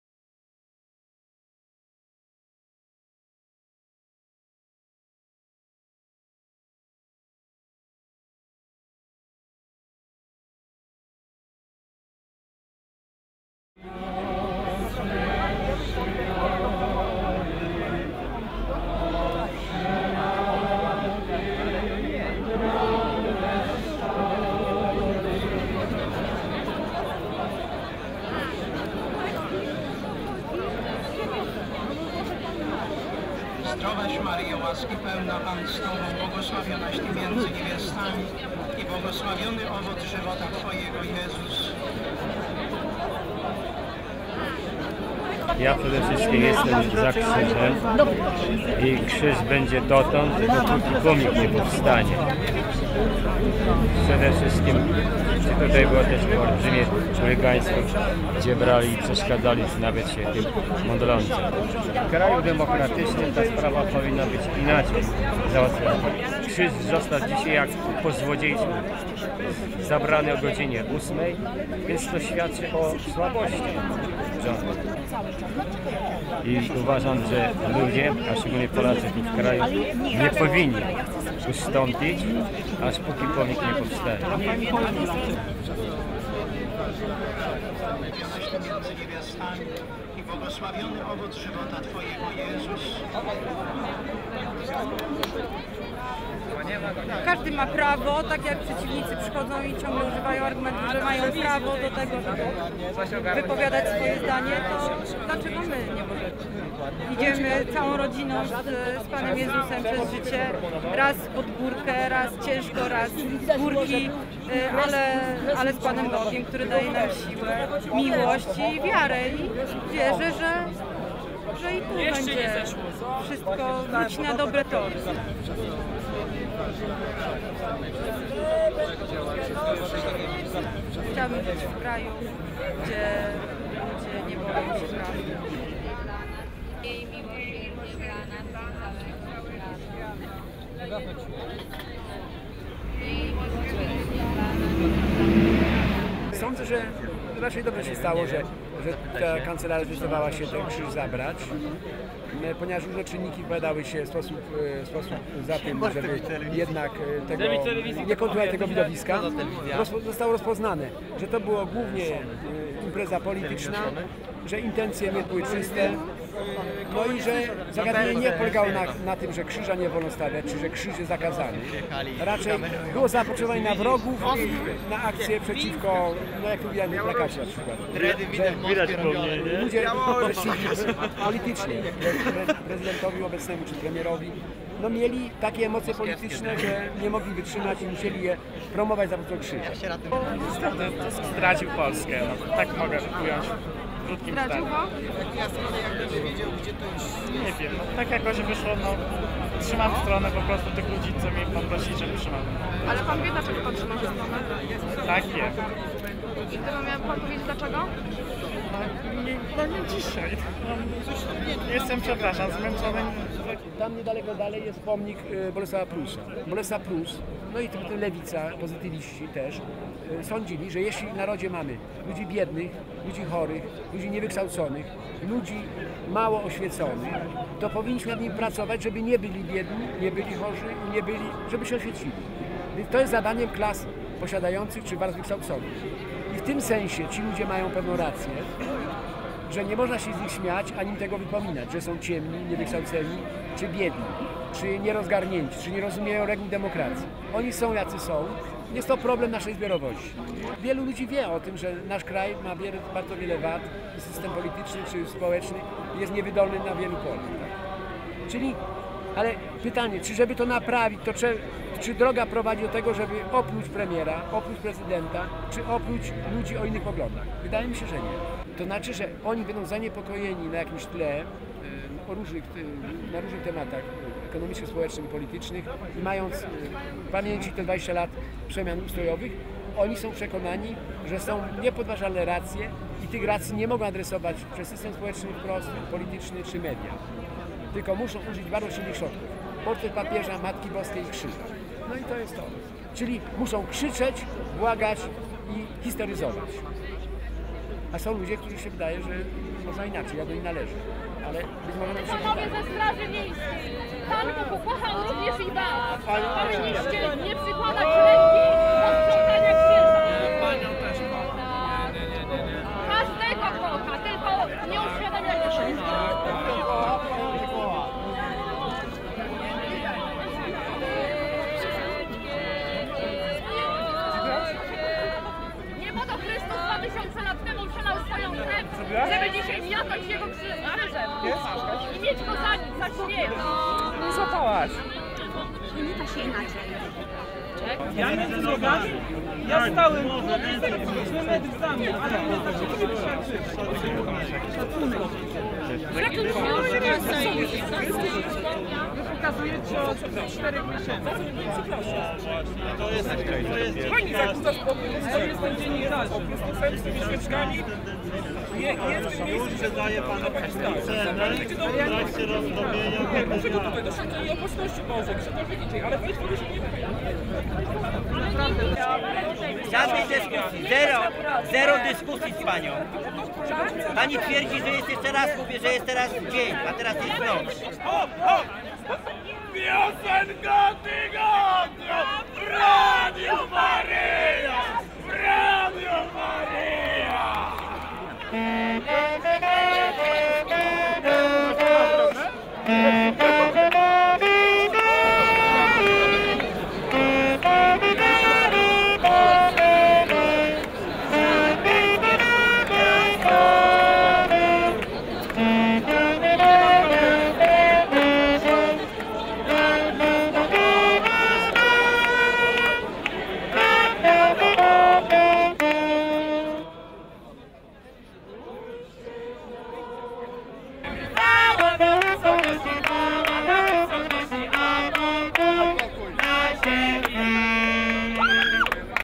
Obraz Polaków pod krzyżem
Materiał zrealizowany w noc po przeniesieniu krzyża umieszczonego przed pałacem prezydenckim podczas żałoby narodowej.